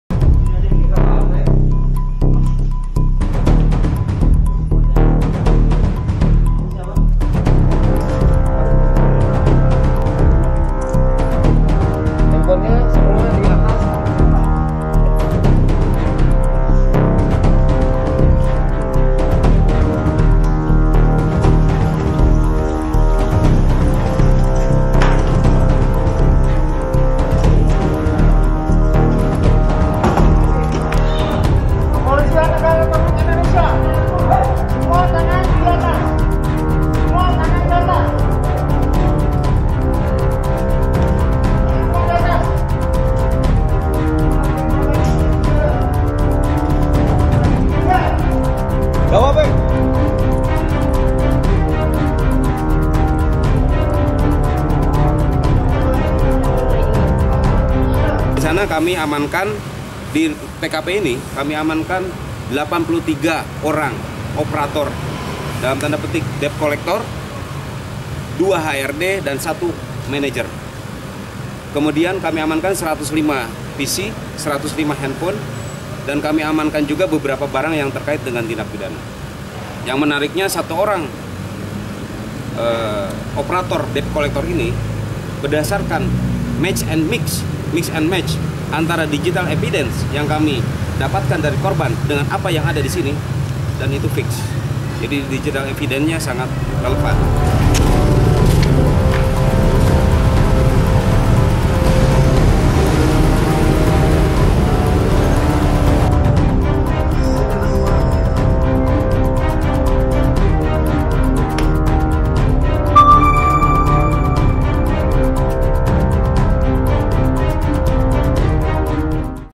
Penggerebekan Kantor Pinjol Yogyakarta, 86 orang Diamankan
Penggerebekan Kantor Pinjaman Online (Pinjol) di Jalan Herman Yohanes, Caturtunggal, Depok, Sleman, Yogyakarta. Penggerebekan dilakukan Kepolisian Ditreskrimsus Polda Jabar, bersama Polda DIY.

Dalam penggrebekan, aparat Kepolsian langsung meminta para operator untuk mengangkat tangan ke atas dan meletakkan telepon genggam di atas meja. Penggerebekan berawal dari laporan salah seorang korban Pinjol ke Polda Jawa Barat.

Korban TM mendapat tekanan yang tidak manusiawi dari pihak Pinjol. Sehingga mengalami depresi dan dirawat di rumah sakit.

Setelah penyelidikan, ternyata operator Pinjol diduga dari kantor di Yogyakarta. Kantor Pinjol tersebut, menggunakan 23 aplikasi yang semuanya tidak terdaftar di OJK. Kasus tersebut, kini dalam penyelidikan Polda Jabar dan rilis resmi akan di sampaikan di Polda Jabar